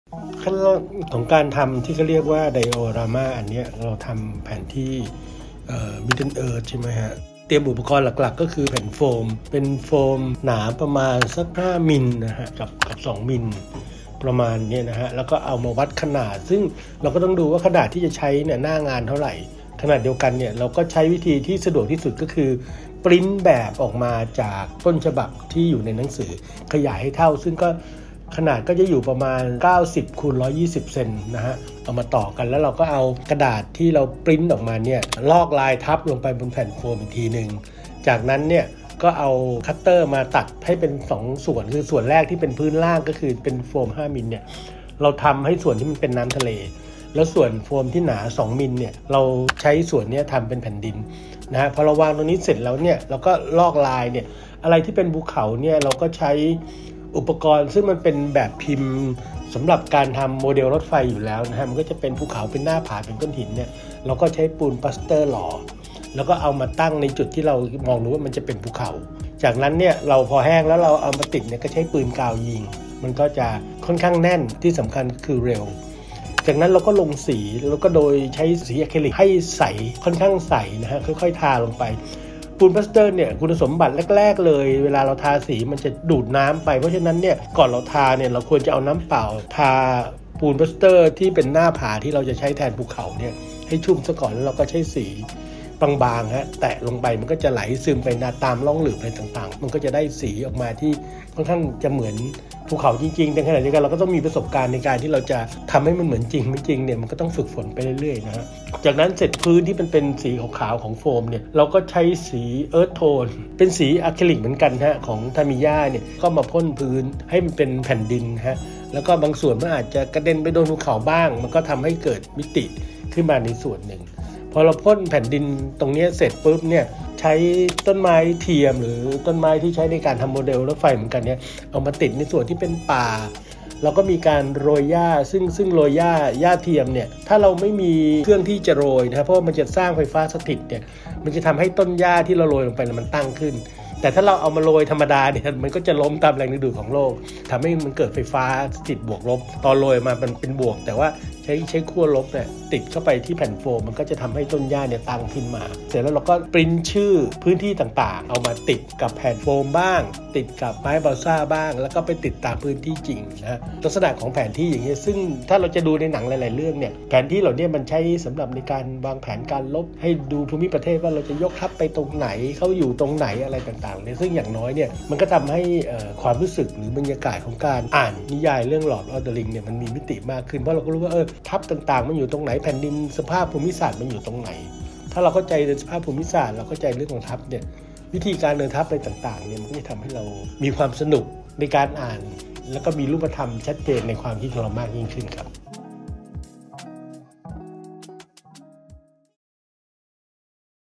ไดโอรามา (แผนที่จำลอง) มิดเดิ้ลเอิร์ธ l The Lord of the Rings
กว่าจะมาเป็นไดโอรามา (แผนที่จำลอง) มิดเดิ้ลเอิร์ธ จากหนังสือเรื่อง The Lord of the Rings ที่ใครหลายคนได้เห็นด้วยตาตนเองไปแล้วจากบู๊ธสำนักพิมพ์แพรวเยาวชนในงานมหกรรมนิยายนานาชาตินั้นไม่ง่ายเลย
วันนี้ทีมงานแพรวเยาวชนจะพาทุกท่านไปชมกระบวนการทั้งหมดตั้งแต่ต้นจบจนพร้อมๆ กัน